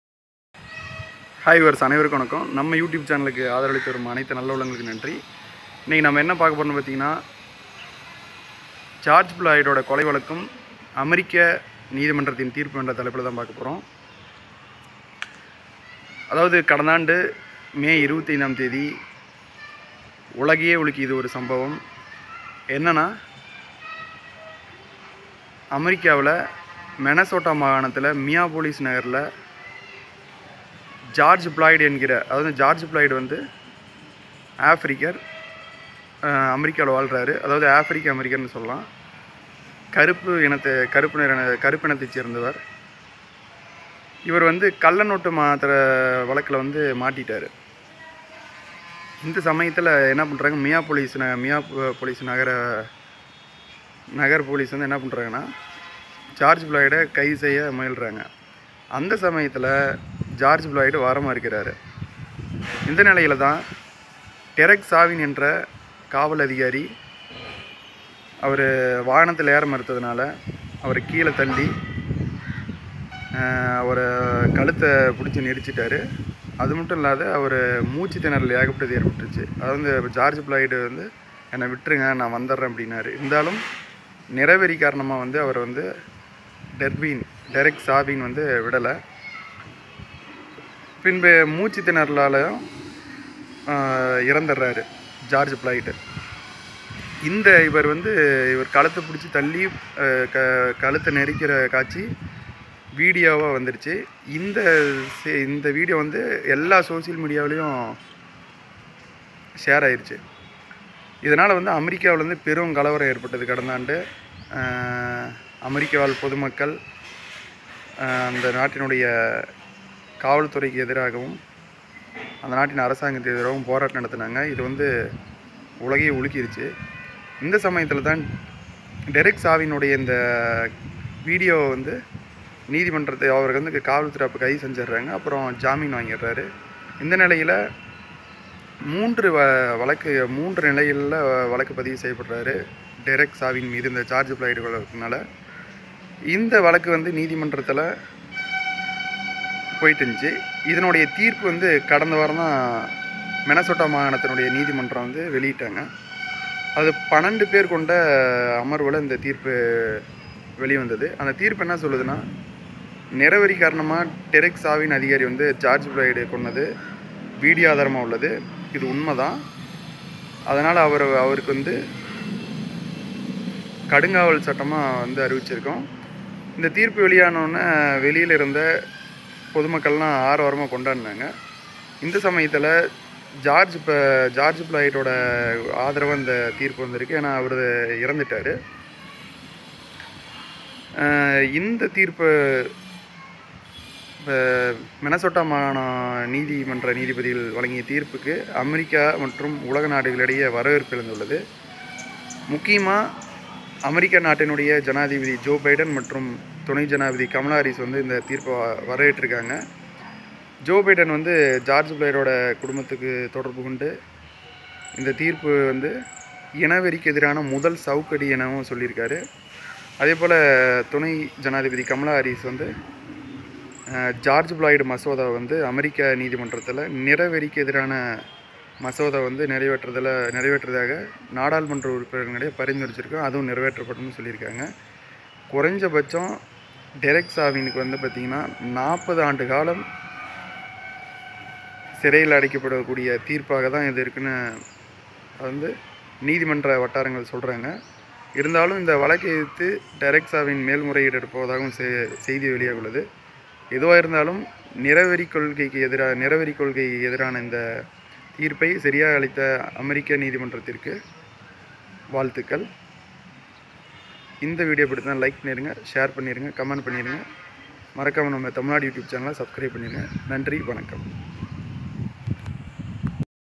ஜார்ஜ் பிளாய்டு கொலை வழக்கும் அமெரிக்கா நீதிமன்ற தீர்ப்பும்| 2020 |2021| மியாபொலிஸ் காவல் அதிகாரி.
ஜார்ஜ் பிளாய்டு கொலை வழக்கும் அமெரிக்கா நீதிமன்ற தீர்ப்பும்.